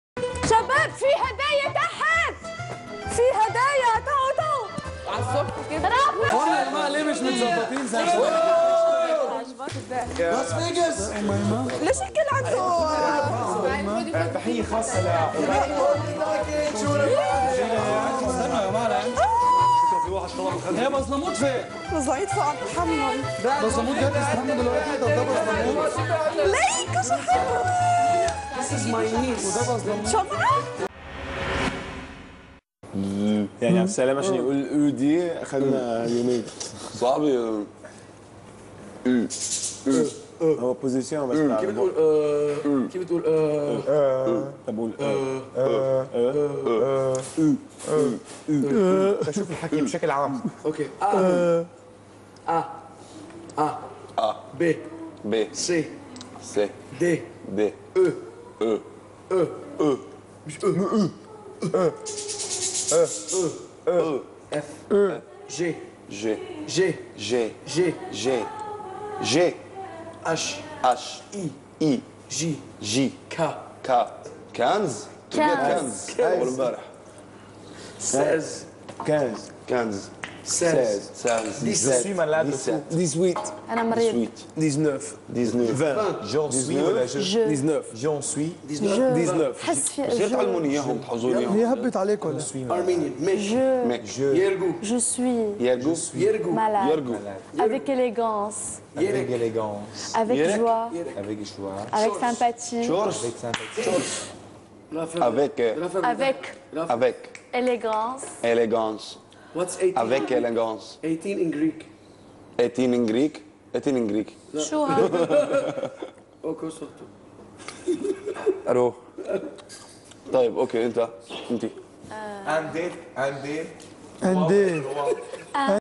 Gilbert Simon
Gilbert Simon teaching his friends French, Armenian and Greek